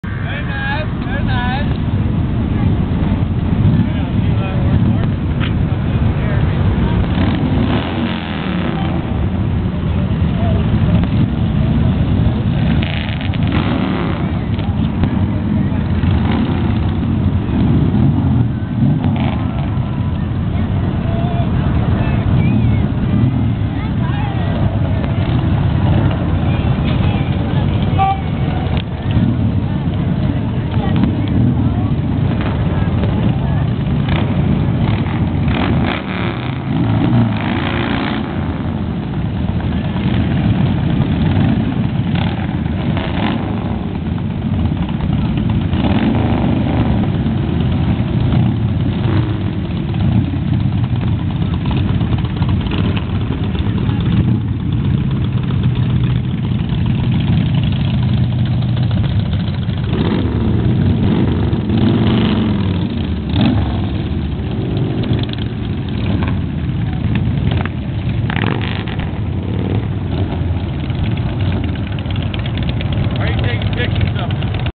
Harley's at the Tempe Veteran's Day Parade
The Harley Davidson motorcycles were in the parade today.